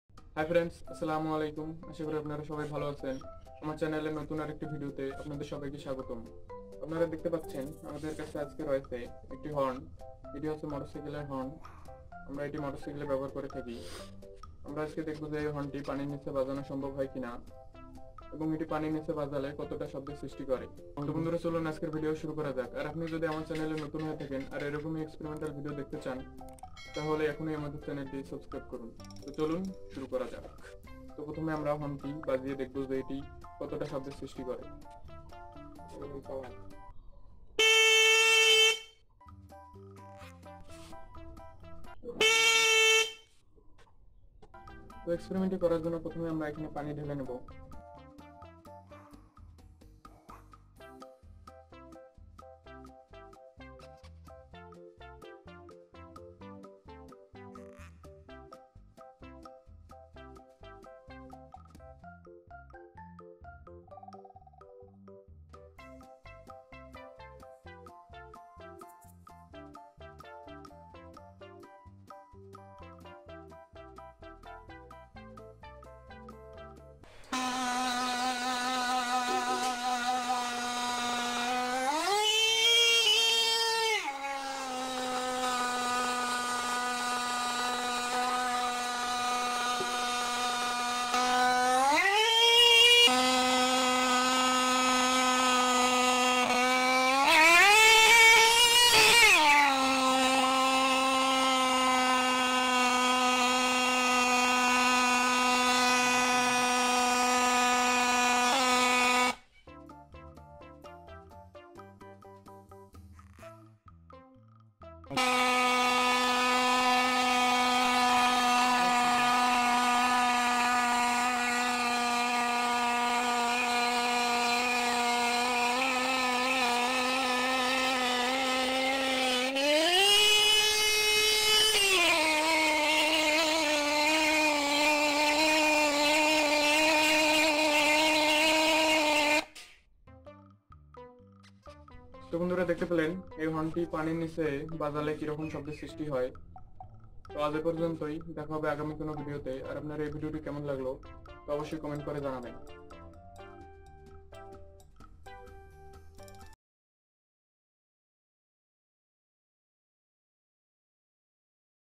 পানির নিচে মোটরসাইকেল হর্ণ বাজালে কি ঘটবে | Experiment with Motorcycle Horne | MR. Bd HACKER [ZAS]
This Video showing পানির নিচে মোটরসাইকেল হর্ণ বাজালে কি ঘটবে?| Experiment with  Motorcycle Horne.

Thanks.

MR. Bd HACKER [ZAS]